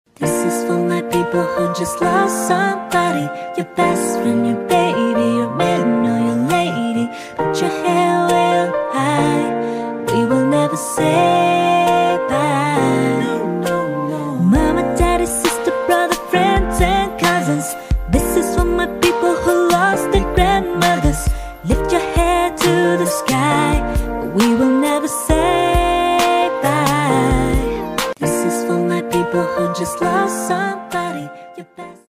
which mica acrylic sheet do you prefer? And welcome to more ideas from you
Lots of people are interested in the production process of acrylic keychains.
So today, let's watch Vograce how to make acrylic keychains.

Vograce is a professional anime peripheral product manufacturer and supplier. IP products, personal artworks, and patterns can be customized. We are located in China.